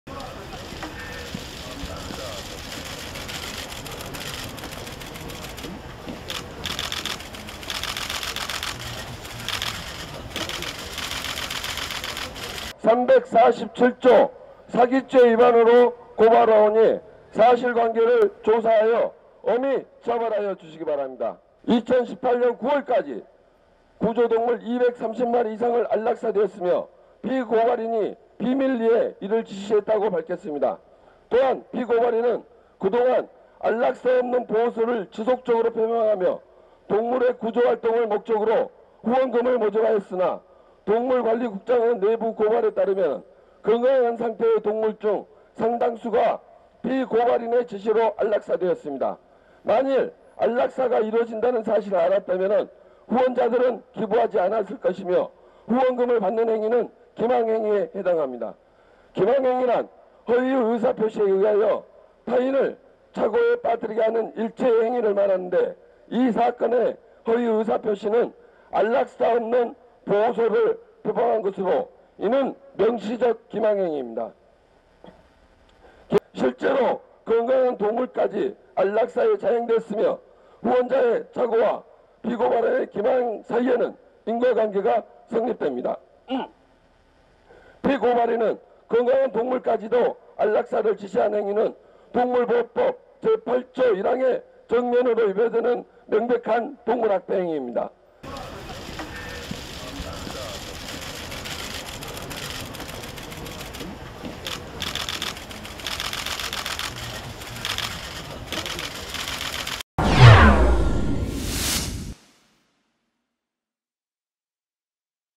'동물 안락사' 케어 박소연 대표 사기 등 혐의로 고발 / 연합뉴스 (Yonhapnews)
(서울=연합뉴스) 시민단체들이 구조동물 안락사 논란을 빚은 동물권단체 '케어'의 박소연 대표를 사기 및 동물보호법 위반 등 혐의로 경찰에 고발했다.
보수 성향 시민단체인 자유연대, 자유대한호국단 등은 18일 서울 종로구 내자동 서울지방경찰청 앞에서 기자회견을 열고 박 대표에 대한 고발장을 접수했다.
이들 단체는 "최근 내부고발자의 폭로에 따르면 박 대표는 2015년부터 지난해까지 구조동물 200마리 이상을 안락사하도록 비밀리에 지시했다"고 주장했다.
이들은 또 "건강한 동물까지도 안락사를 지시한 행위는 동물보호법 제8조 제1항에 정면으로 위배되는 명백한 동물 학대 행위"라고 주장했다.
한편 비글구조네트워크와 동물과함께행복한세상 등 동물보호 단체들도 이날 오후 4시 박 대표에 대한 고발장은 서울중앙지검에 제출할 계획이다.